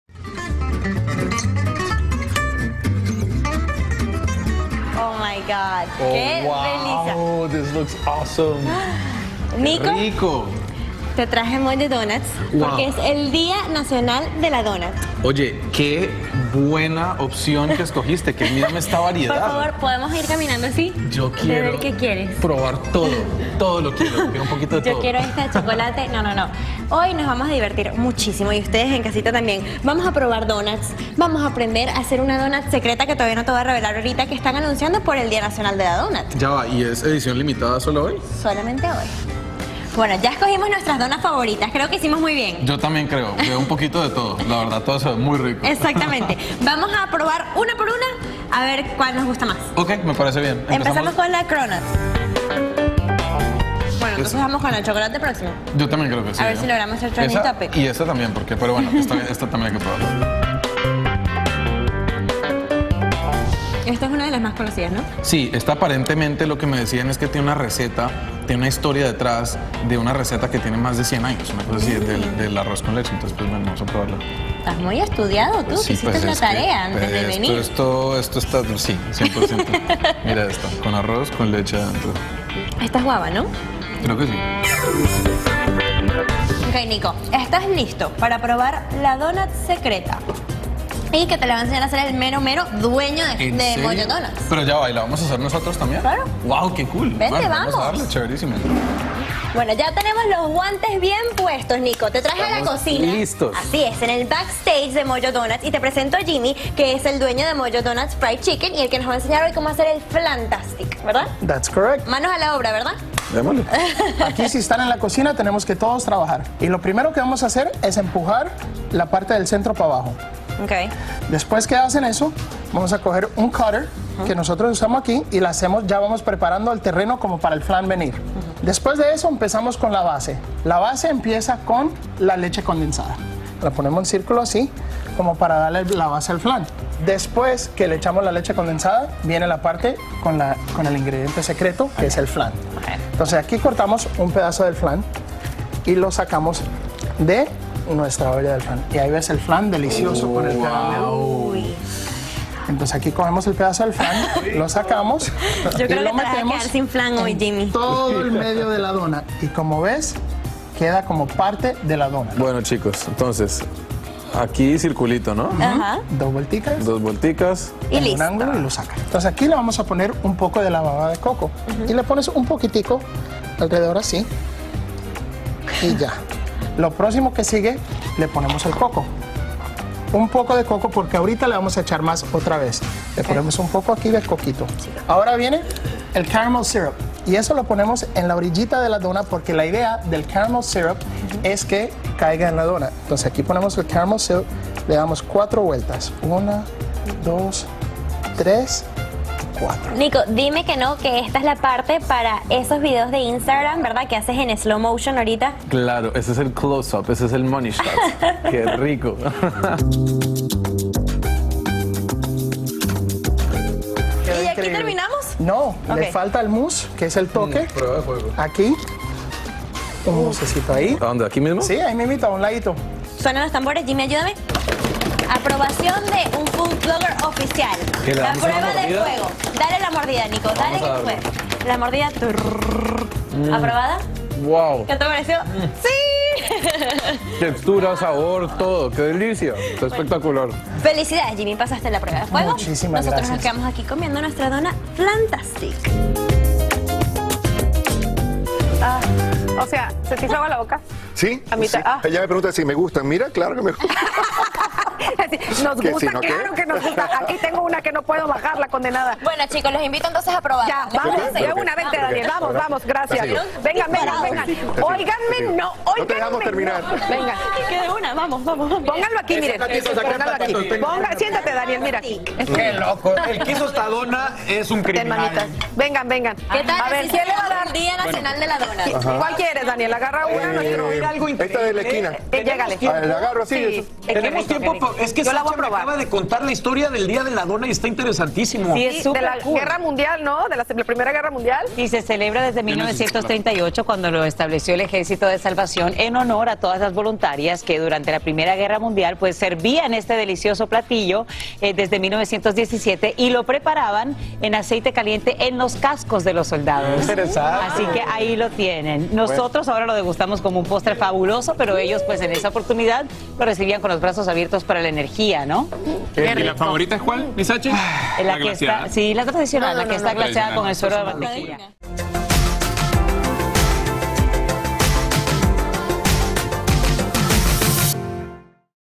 Por el Día Nacional de la Dona, probamos una rellena de flan con caramelo
El National Doughnut Day es digno de celebrarse por todo lo alto, por eso nuestra Jessi Rodríguez invitó al 'food influencer' Nico Noreña a descubrir esta novedosa receta de una dona rellena de flan. Y por supuesto que no íbamos a quedarnos sin probarla en el estudio.